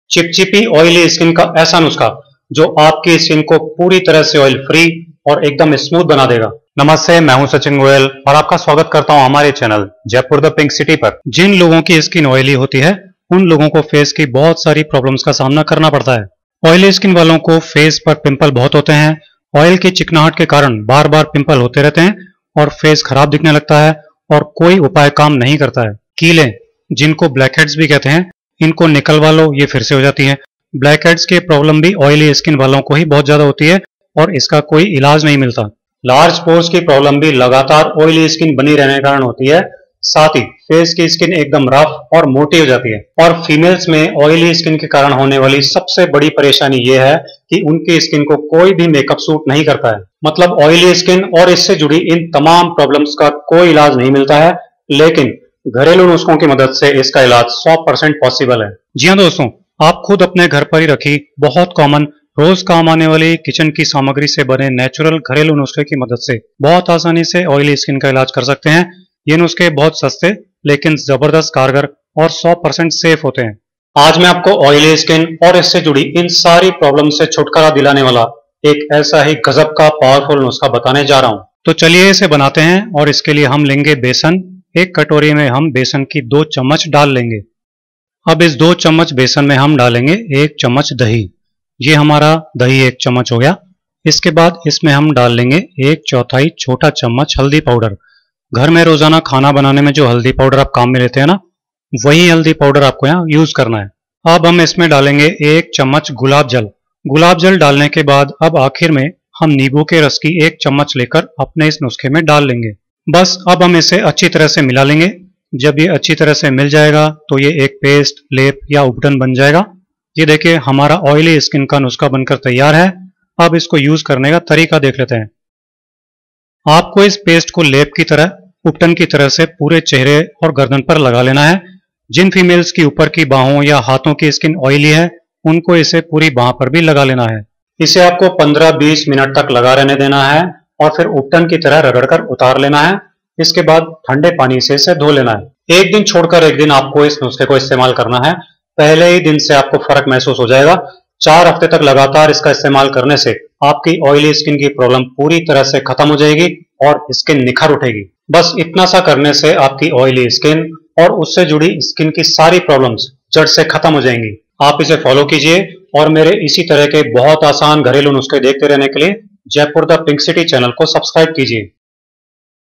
ऑयली स्किन के लिए घरेलू उपाय - oily skin ke liye kya karen - oily skin remedy in Hindi by Sachin
ऑयली स्किन के लिए घरेलू उपाय - oily skin ke liye kya karen - oily skin remedy in Hindi by Sachin Goyal

दोस्तों, आज के इस वीडियो में आप देखेंगे कि ऑयली स्किन क्या होती है, ऑयली स्किन क्यों होती है, ऑयली स्किन के लक्षण क्या होते हैं, ऑयली स्किन के कारण क्या होते हैं और ऑयली स्किन के घरेलू उपचार क्या हैं जिनसे आप घर पर ही रसोई में ही मौजूद चीजों से बड़ी ही आसानी से ऑयली स्किन की दवा बना कर अपने ऑयली स्किन का इलाज कर सकते हैं.

doston, aaj ke is video mein aap dekhenge ki oily skin kya hoti hai, oily skin kyon hoti hai, oily skin ke lakshan kya hote hain, oily skin ke karan kya hote hain, aur oily skin ke gharelu upchar kya hain jinse aap ghar par hi rasoi me hi maujood chijo se badi hi aasani se oily skin ki dawa banakar apne oily skin ka ilaj kar sakte hain.

Friends, in today's video you will see what is oily skin, why oily skin happens, what are the symptoms of oily skin, what are the causes of oily skin, and an easy home remedy for oily skin treatment with the help of kitchen ingredients.

jaipurthepinkcity channel ke sabhi gharelu nuskhe dekhne ke liye is link ko click kijiye aur apni har health problem ghar baithe hi solve kijiye